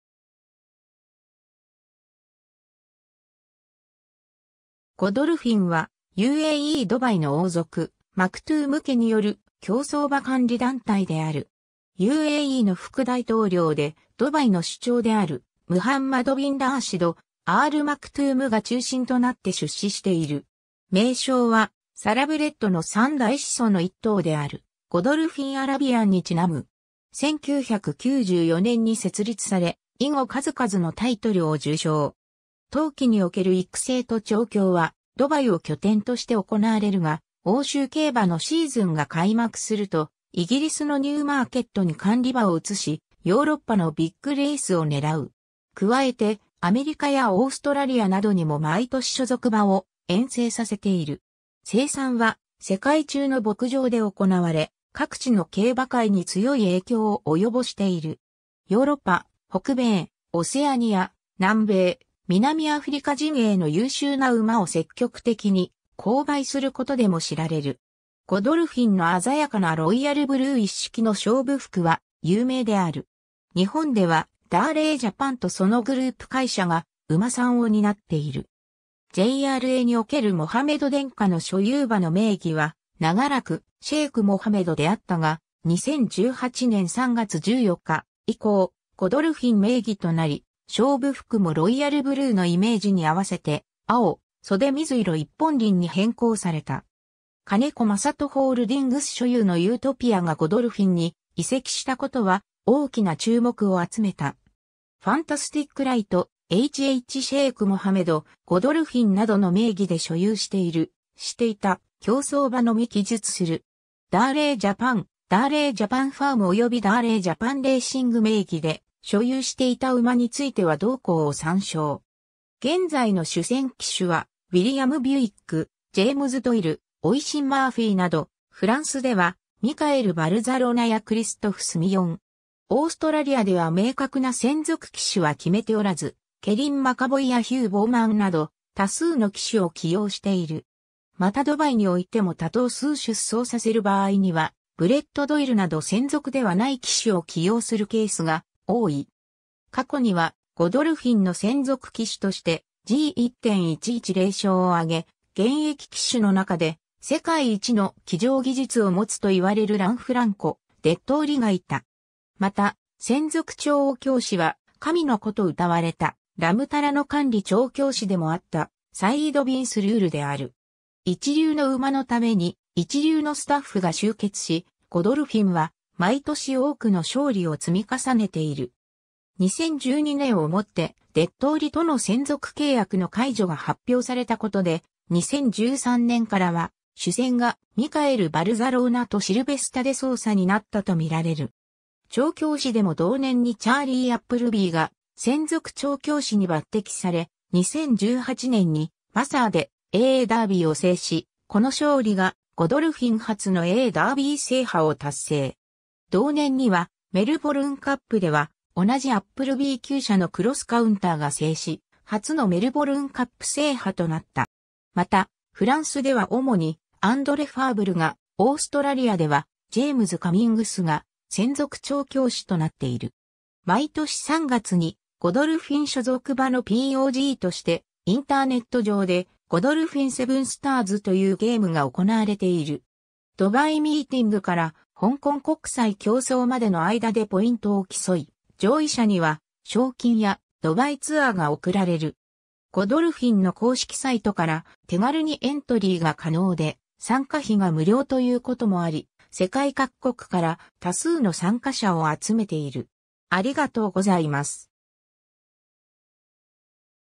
ゴドルフィン
ゴドルフィン（Godolphin Racing Inc.）は、UAE・ドバイの王族マクトゥーム家による競走馬管理団体である。
UAEの副大統領でドバイの首長であるムハンマド・ビン＝ラーシド・アール＝マクトゥーム（しばしばシェイク・モハメドと表記される）が中心となって出資している。
名称はサラブレッドの三大始祖の一頭であるゴドルフィンアラビアンにちなむ。
 1994年に設立され、以後数々のタイトルを受賞。
冬季における育成と調教はドバイを拠点として行われるが、欧州競馬のシーズンが開幕するとイギリスのニューマーケットに管理馬を移し、ヨーロッパのビッグレースを狙う。
加えて、アメリカやオーストラリアなどにも毎年所属馬を遠征させている。
 生産は世界中の牧場で行われ、各地の競馬界に強い影響を及ぼしている。
ヨーロッパ(イギリス・アイルランド・ドイツ・フランス・イタリア)・北米(アメリカ・カナダ)・オセアニア(オーストラリア)・南米(主にアルゼンチン・ブラジル・チリ・ウルグアイ)・南アフリカ陣営の優秀な馬を積極的に購買することでも知られる。
 ゴドルフィンの鮮やかなロイヤルブルー一色の勝負服は有名である。
 日本ではダーレー・ジャパンとそのグループ会社が馬産を担っている。
JRAに於けるモハメド殿下の所有馬の名義は長らく「シェイク・モハメド」であったが、2018年3月14日以降「ゴドルフィン」名義となり、勝負服もロイヤルブルーのイメージに合わせて「青・袖水色1本輪」に変更された。
金子真人ホールディングス所有のユートピアがゴドルフィンに移籍したことは大きな注目を集めた。
 ファンタスティックライト(ゼッケン10番) 「H.H.シェイク・モハメド」「ゴドルフィン」などの名義で所有している / していた競走馬のみ記述する。
ダーレー・ジャパン、ダーレー・ジャパン・ファーム及びダーレー・ジャパン・レーシング名義で所有していた馬については同項を参照。
 現在の主戦騎手はウィリアム・ビュイック、ジェームズ・ドイル、オイシン・マーフィーなど、フランスではミカエル・バルザローナやクリストフ・スミヨン。
オーストラリアでは明確な専属騎手は決めておらず、ケリン・マカヴォイやヒュー・ボウマンなど多数の騎手を起用している。
またドバイにおいても多頭数出走させる場合にはブレット・ドイルなど専属ではない騎手を起用するケースが多い。
過去にはゴドルフィンの専属騎手として、G1・110勝を挙げ、現役騎手の中で世界一の騎乗技術を持つといわれるランフランコ・デットーリがいた。
また、専属調教師は神の子と謳われたラムタラの管理調教師でもあったサイード・ビン・スルールである。
一流の馬のために一流のスタッフが集結し、ゴドルフィンは毎年多くの勝利を積み重ねている。
2012年をもってデットーリとの専属契約の解除が発表されたことで、2013年からは主戦がミカエル・バルザローナとシルヴェスタ・デソウサになったとみられる。
調教師でも同年にチャーリー・アップルビーが専属調教師に抜擢され、2018年にマサーで英ダービーを制し、この勝利がゴドルフィン初の英ダービー制覇を達成。
同年にはメルボルンカップでは同じアップルビー厩舎のクロスカウンターが制し、初のメルボルンカップ制覇となった。
またフランスでは主にアンドレ・ファーブルが、オーストラリアではジェームズ・カミングスが専属調教師となっている。
 毎年3月にゴドルフィン所属馬のPOGとして、インターネット上でゴドルフィンセブンスターズ（Godolphin Seven Stars Competition）というゲームが行われている。
ドバイミーティングから香港国際競走までの間でポイントを競い、上位者には賞金やドバイツアーが贈られる。